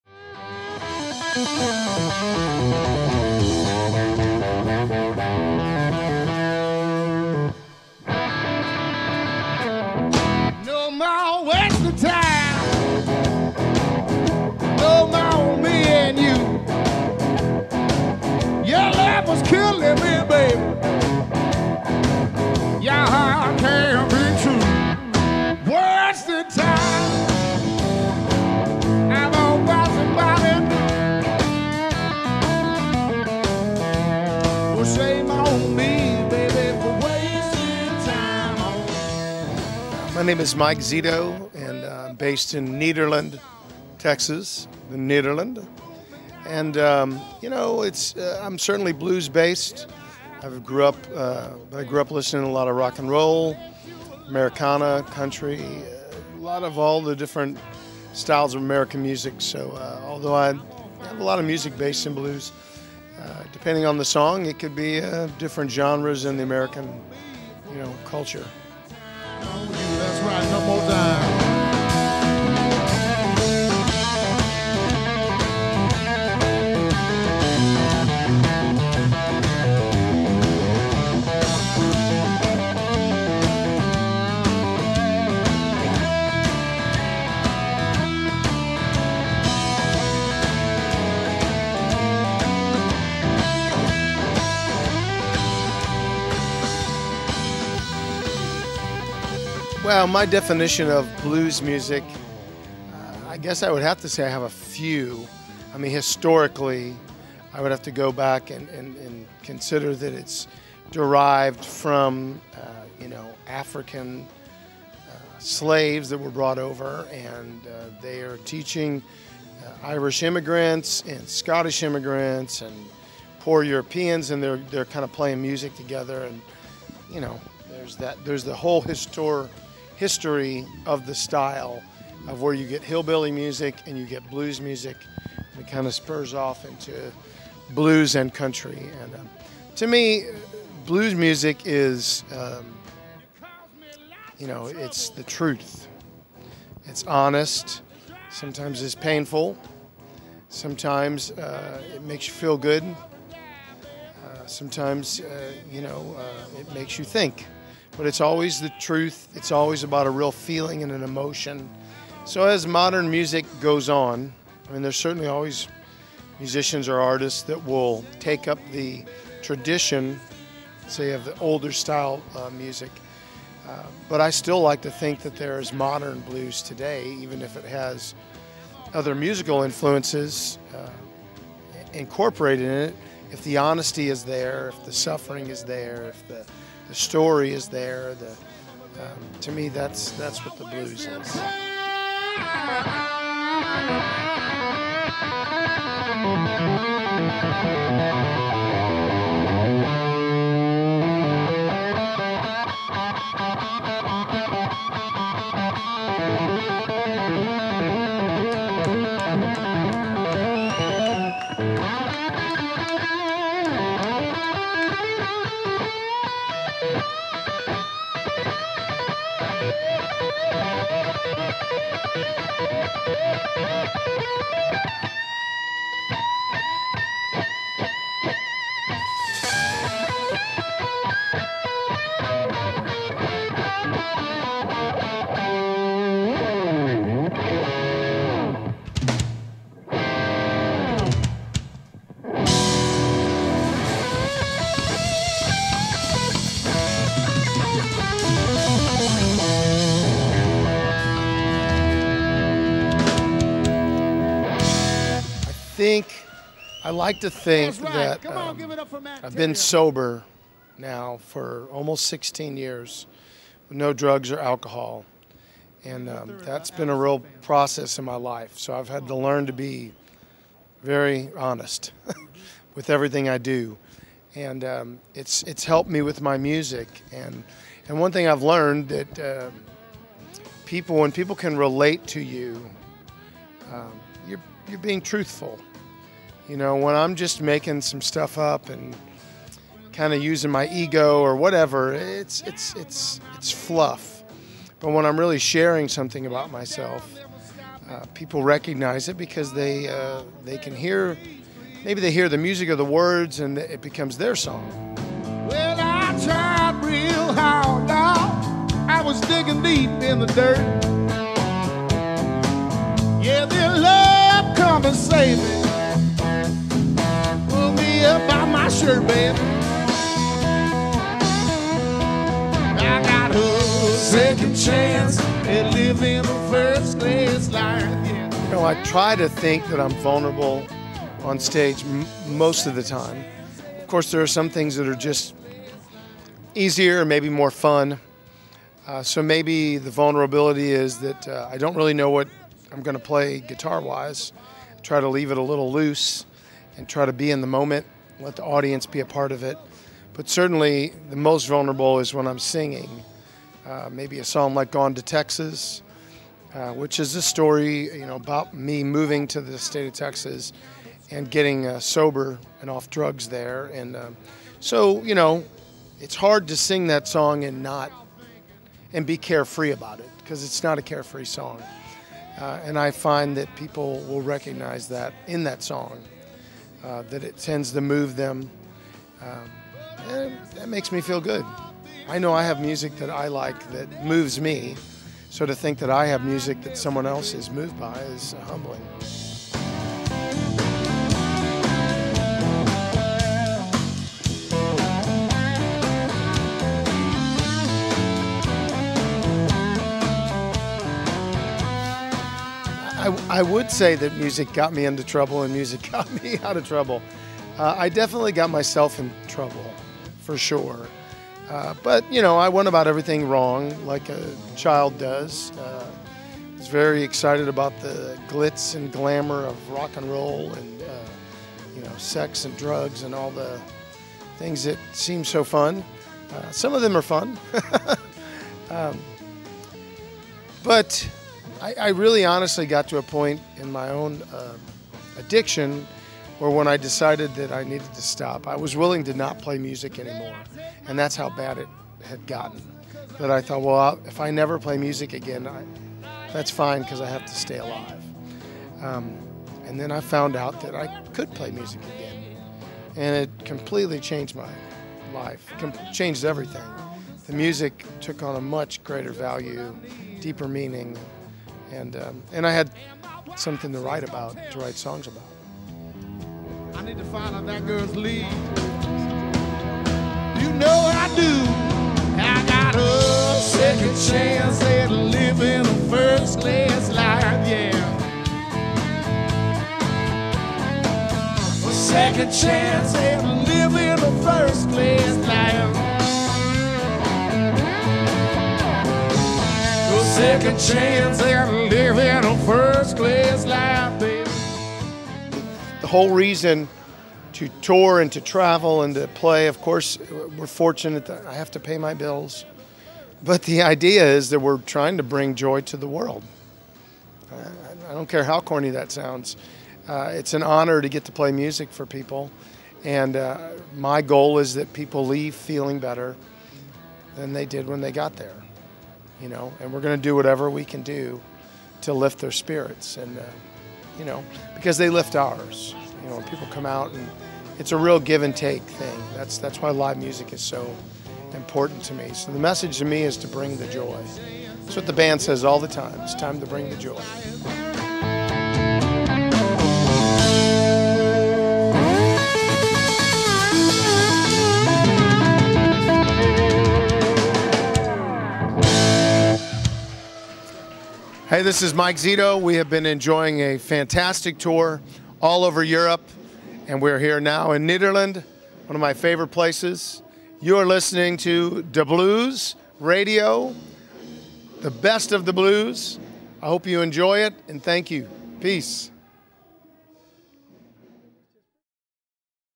Mike Zito - interview and impression of show - De FLux (NL)
During the last day of his extensive tour through Europe we talked with Mike Zito about the blues, the addictions he overcame and his deep love for the music.